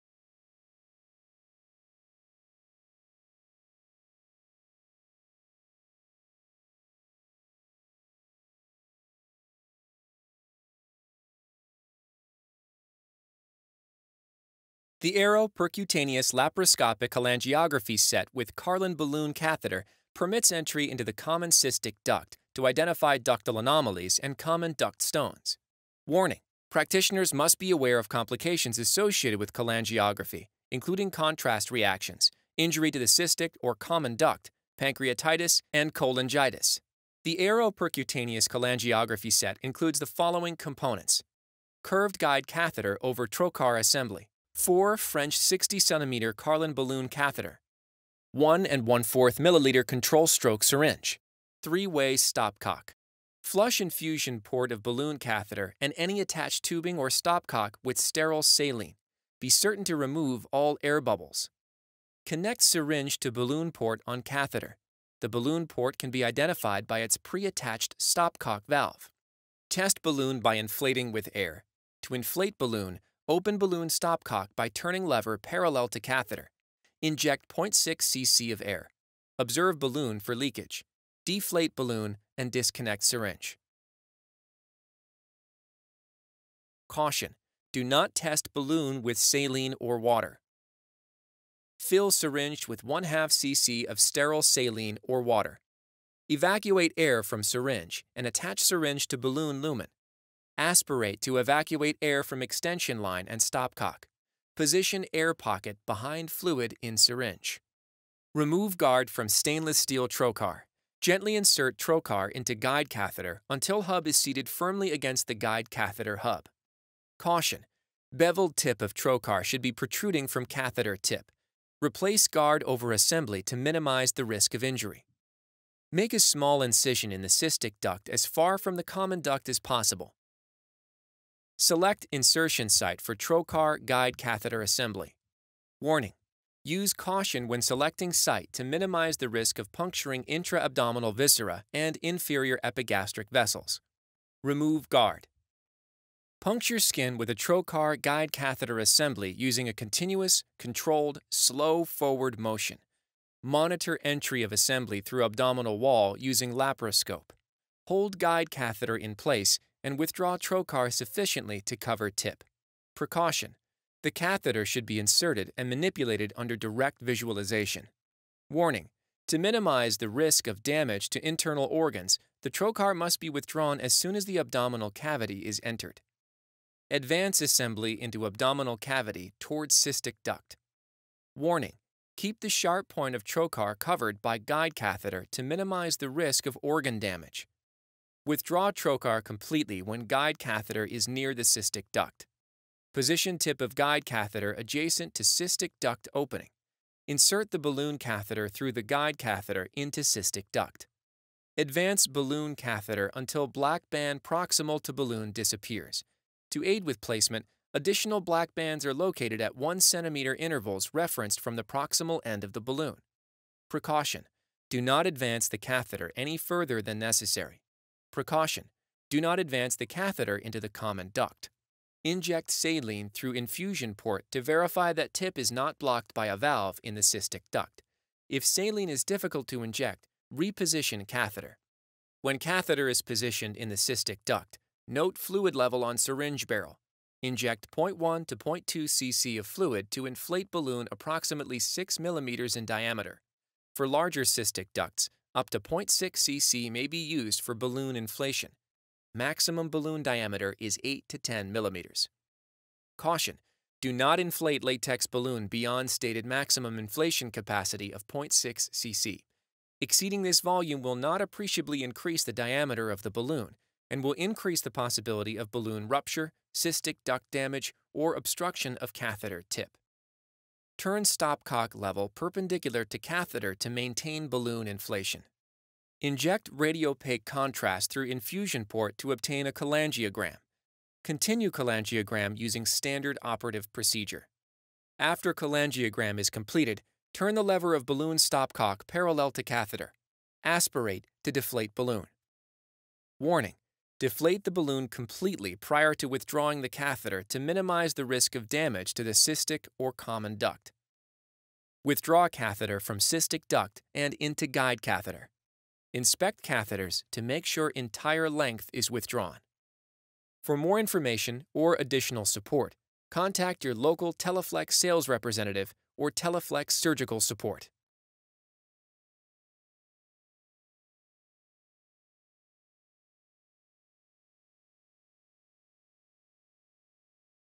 Arrow-Karlan Laparoscopic Balloon Catheter SU Set with Balloon Catheter In-service with insertion
This in-service instructional video shows step by step instructions and proper use warnings of the Arrow-Karlan Laparoscopic Balloon Catheter SU Set with Balloon Catheter with lab-performed insertion example